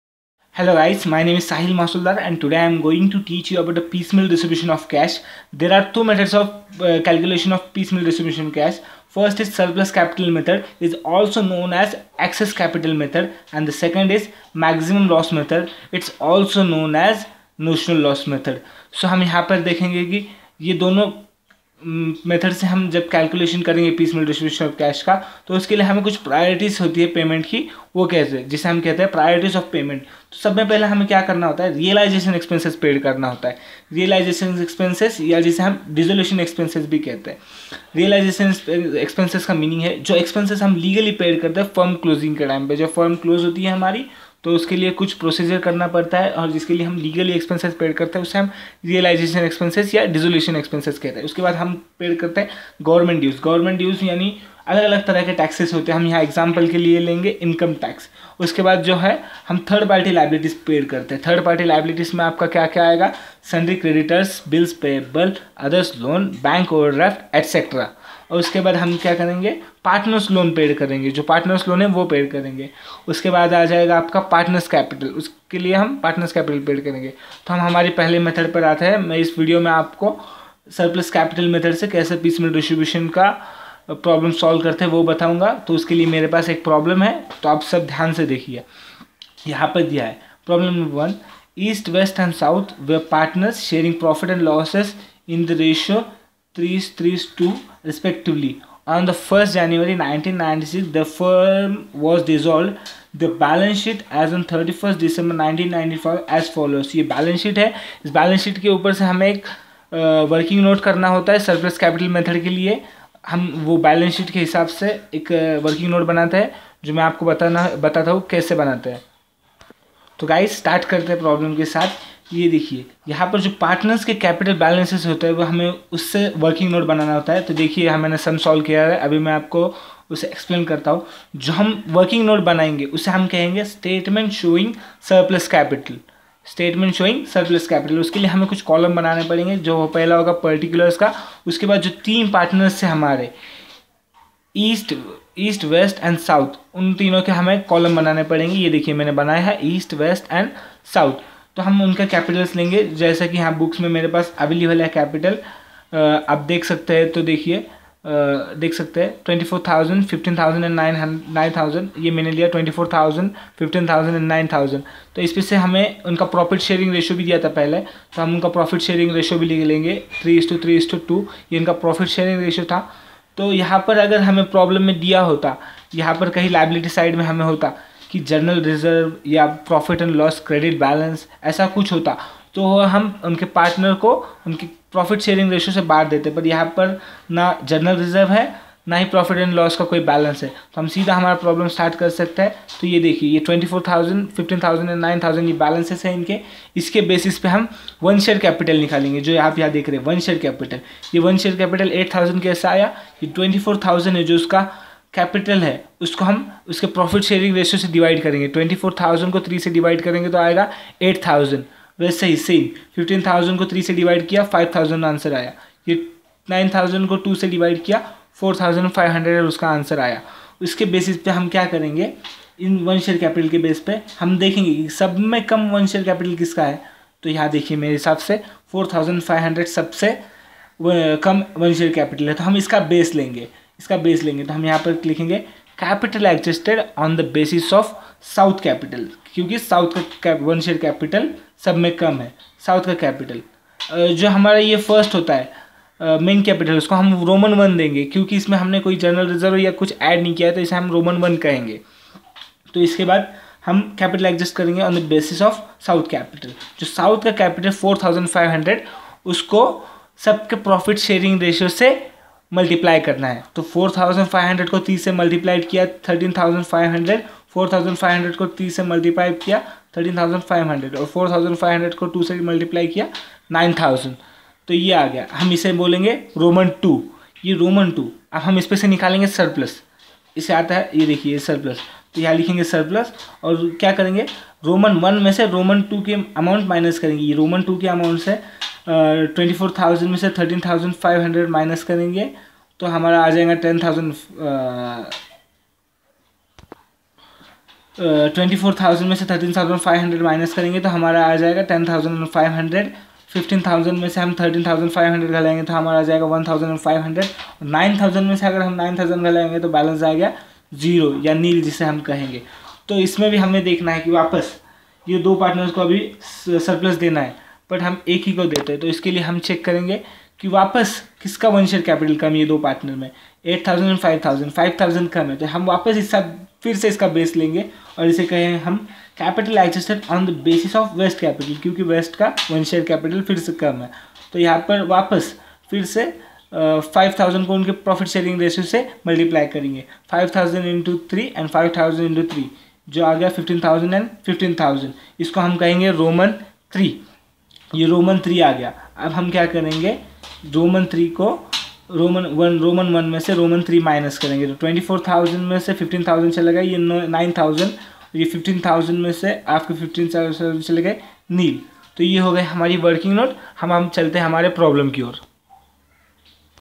Introduction to Piecemeal Distribution of cash as per surplus capital method
Statement showing surplus capital and introduction to piecemeal distribution of cash and various methods of solving the sum ...